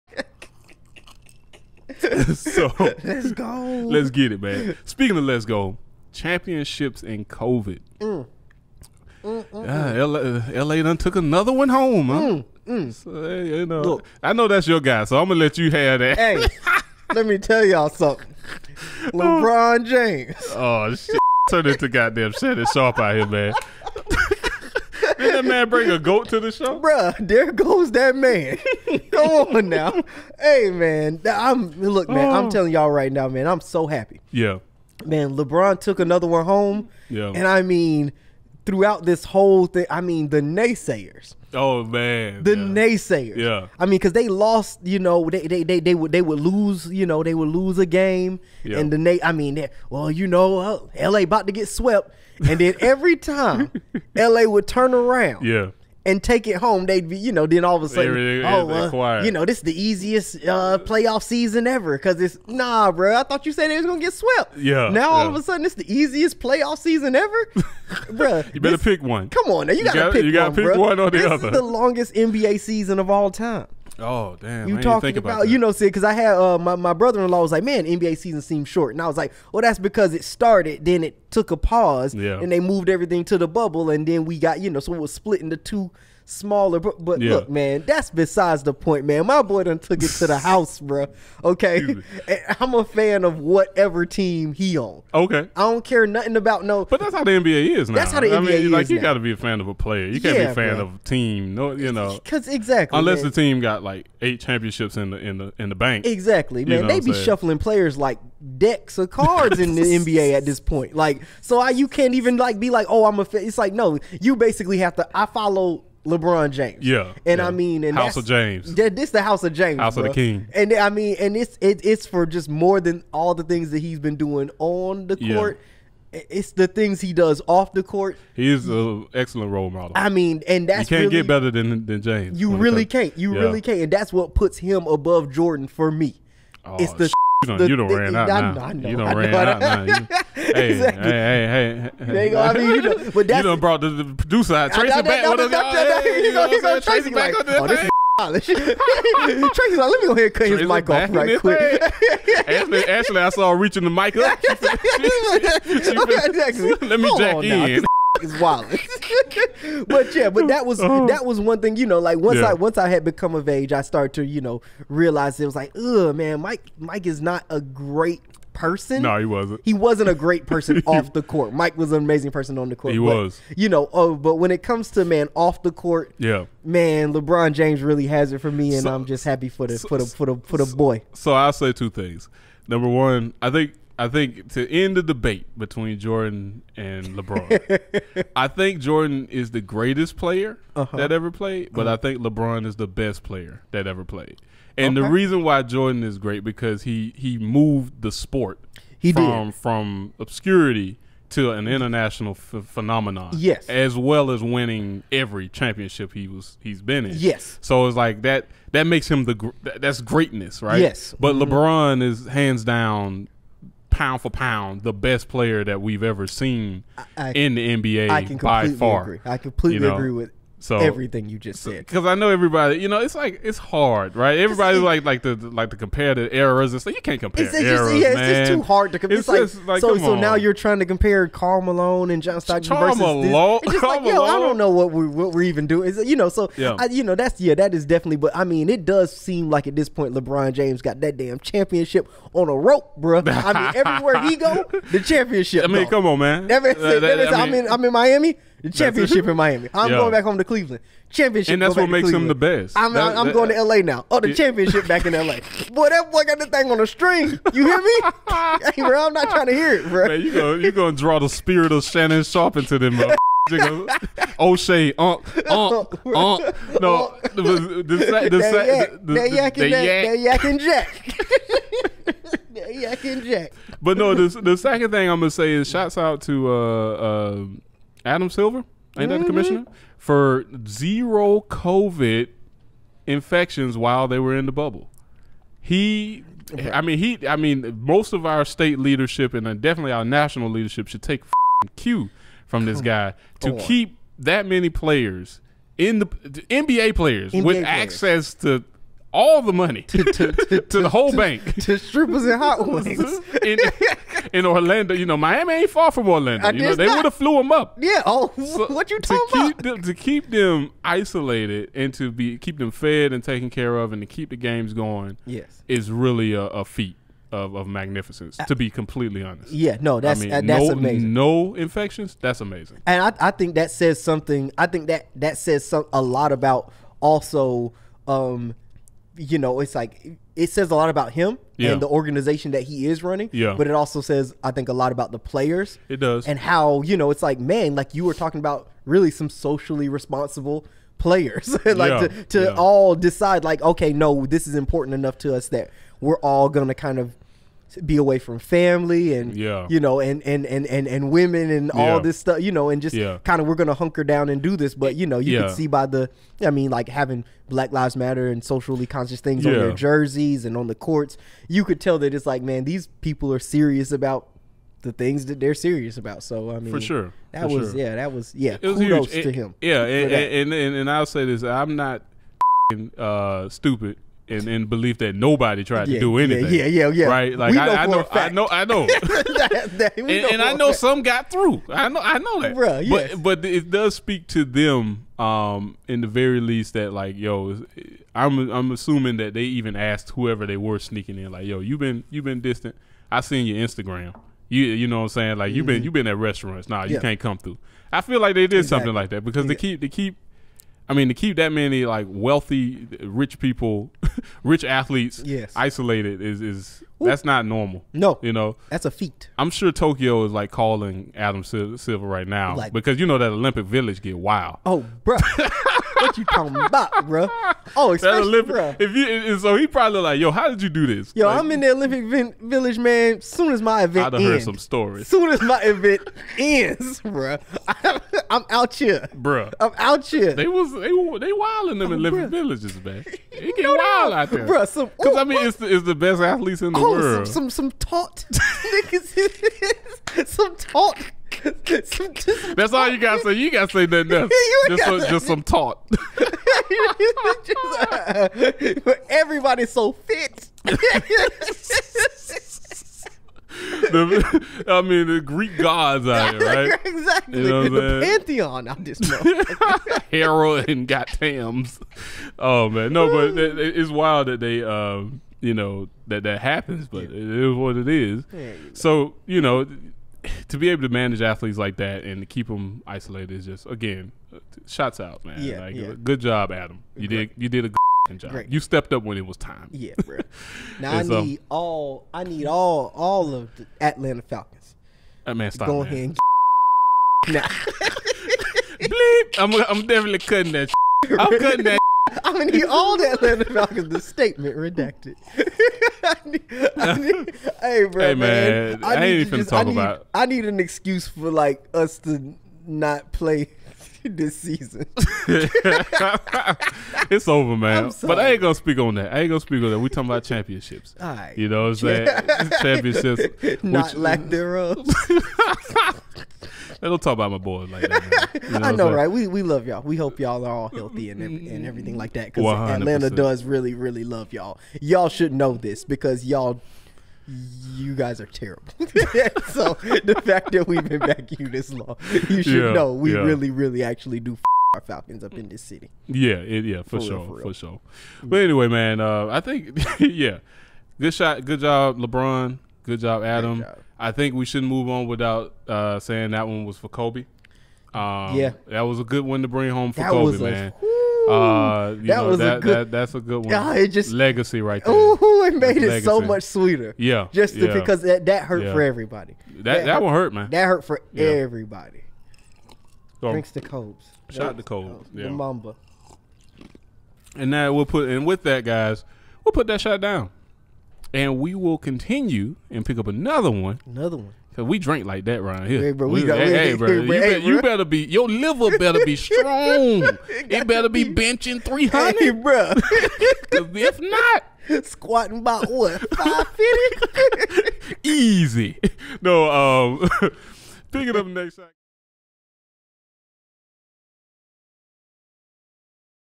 Drink of the Day: Lebron & The Lakers Take Home The Championship
Your bartenders Nick & Tobias give their takes and elation for the results of the NBA Finals!